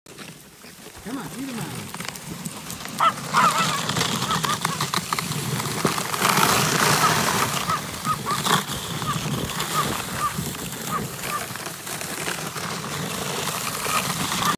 Dogs Drag Sled and Baby Sitting on it - 1361302
These yorkeranian sled dogs drew the sled as they ran enthusiastically through the snow and also pulled along the baby sitting on the sled.